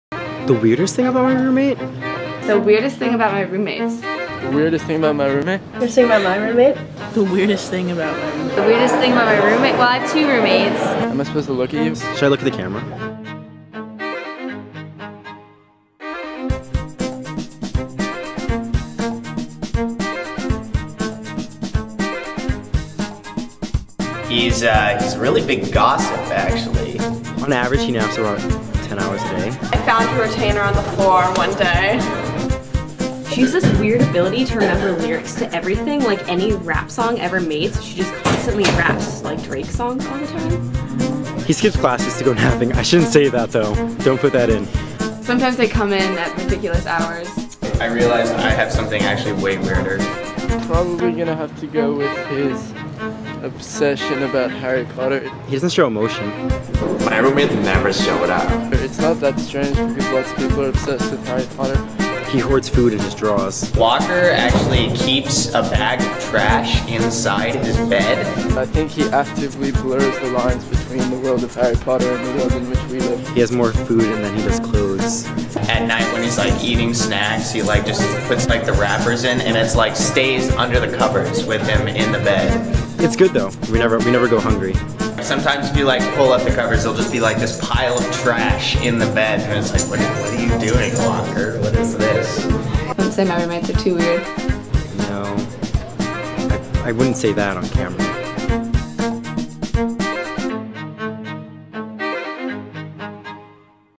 Roommates
Freshmen talk about their roommates and all down right "weird" things they do!

Part of the "Fresh Impressions '15" series